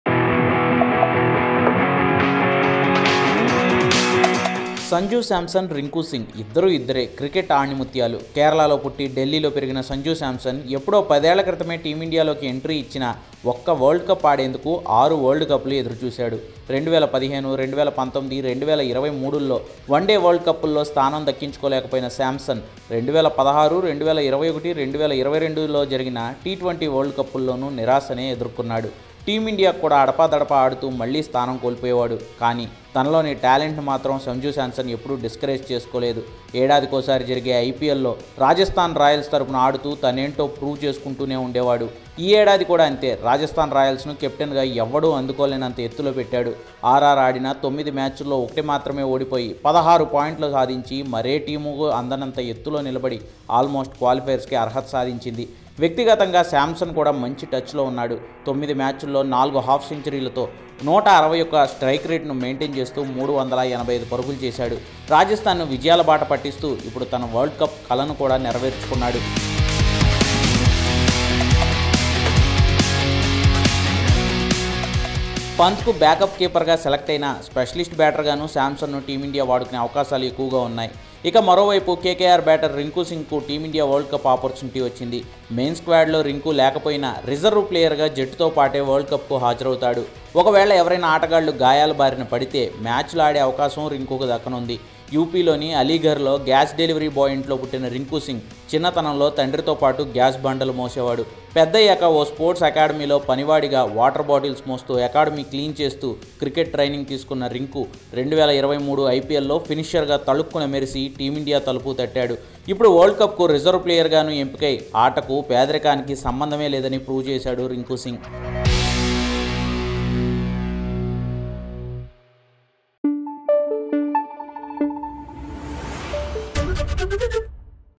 Sanju Samson Rinku Singh T20 World Cup 2024 | టీ20 వరల్డ్ కప్ జట్టులో సంజూ, రింకూ | ABP Desam
సంజూ శాంసన్, రింకూ సింగ్ ఇద్దరూ ఇద్దరే క్రికెట్ ఆణిముత్యాలు. కేరళలో పుట్టి ఢిల్లీలో పెరిగిన సంజూ శాంసన్ ఎప్పుడో పదేళ్ల క్రితమే టీమిండియాలోకి ఎంట్రీ ఇచ్చినా ఒక్క వరల్డ్ కప్ ఆడేందుకు...ఆరు వరల్డ్ కప్పులు ఎదురు చూశాడు. రింకూ సింగ్ అయితే పాచిపని చేసుకుంటూ ఈ స్థాయికి ఎదిగాడు.